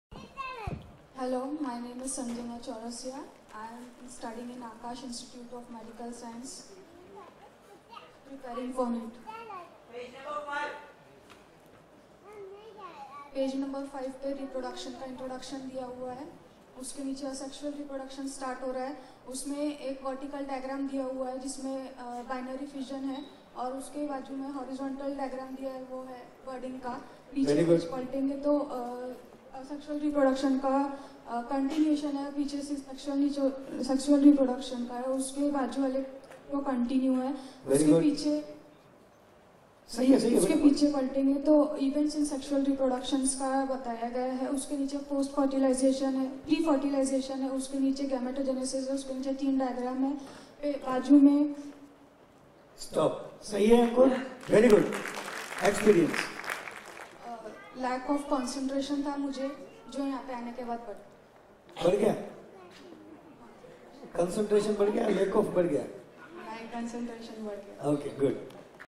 Demo After Dharmesh Pithva's Training By Sanjana Chourasia Nagpur 2017
Dharmesh Pithva's Magic of Memory  Workshop @ Nagpur-2017

Dr. vasantrao deshpande auditorium - Civil Lines, Nagpur, Maharashtra &
vidarbha hindi sahitya sammelan - Jhansirani Square, Sitabuldi, Nagpur, Maharashtra